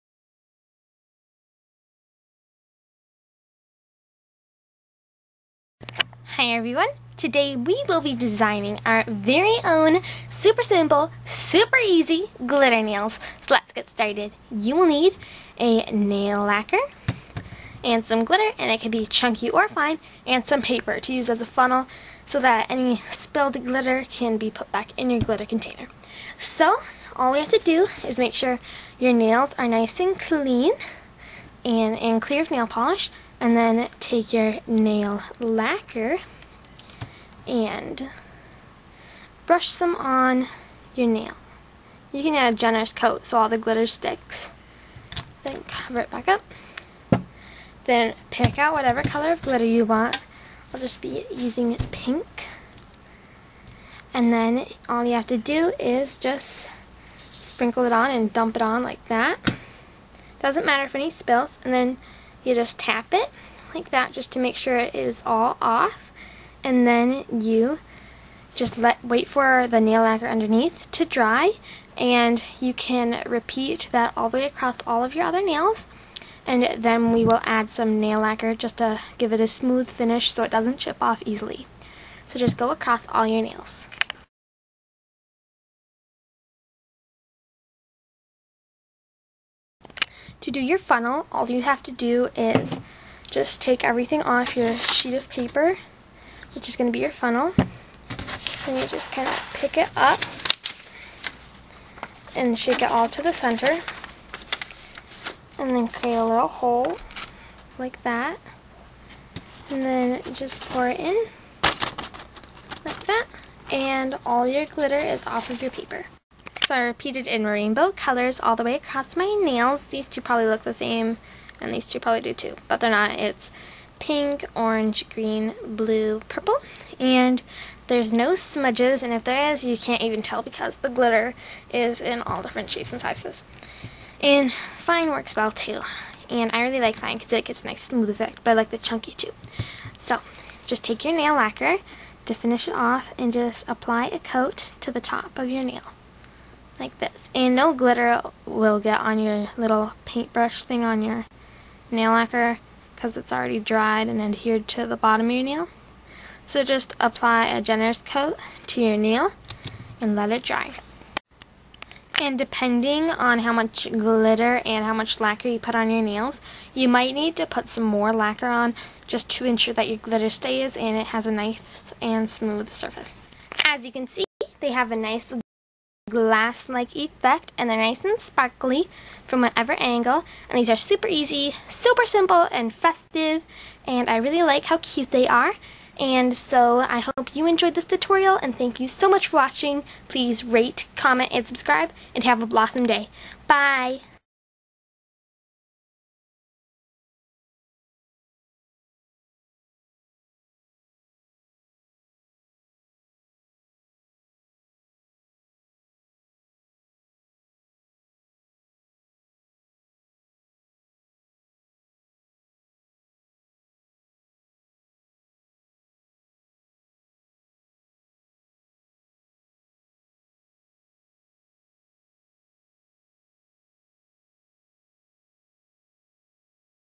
~DIY GLITTER NAILS!~
Hi everyone! Today we will be designing our very own glitter nails. I hope you enjoy. Have a blossom day, bye!